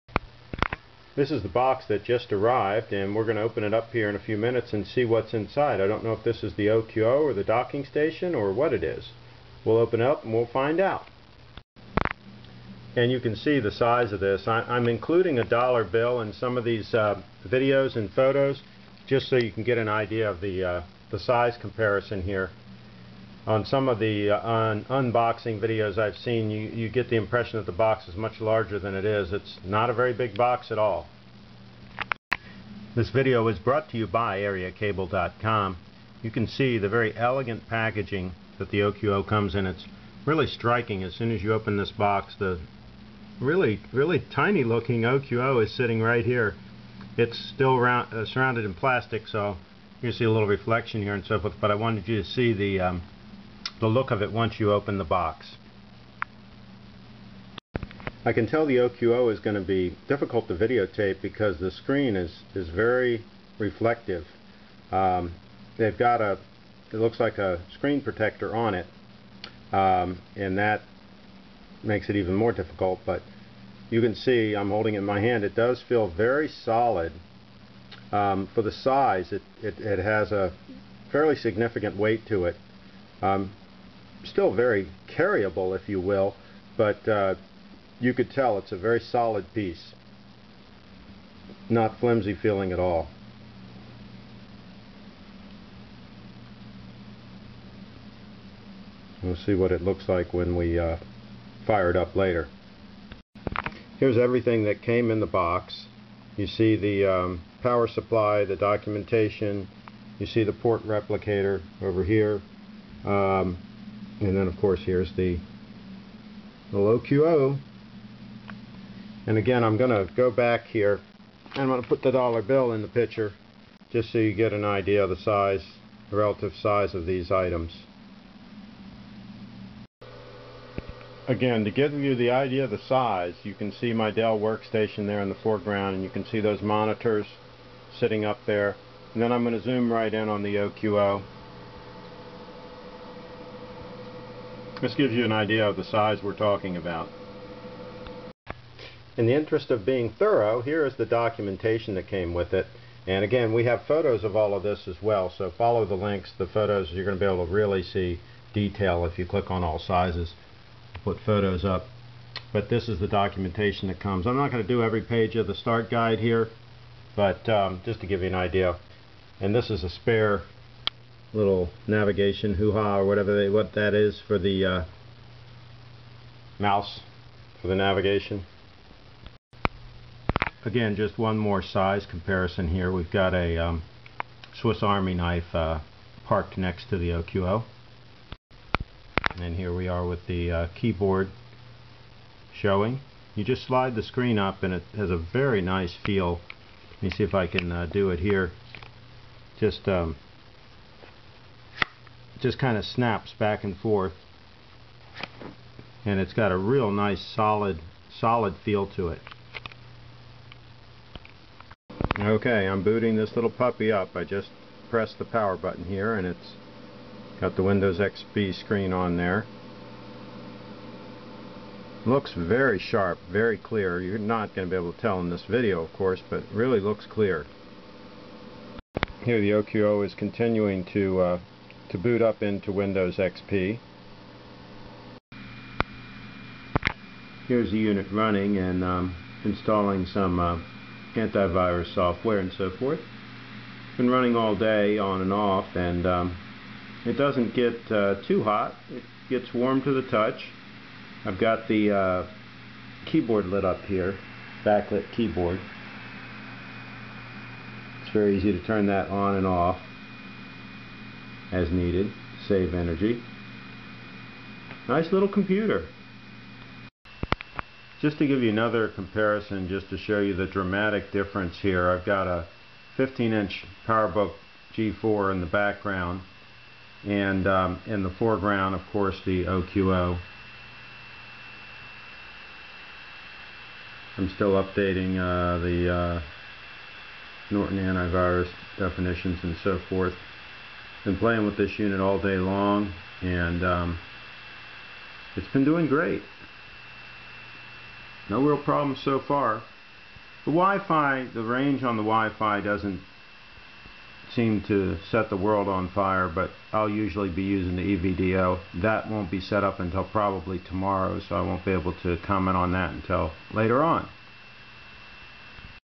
OQO - Model 02, Micro Computer
Take a look at the unboxing on the tiny OQO Model 02 Computer. Go to Frederick.com keyword "OQO" for my article with many huge photos!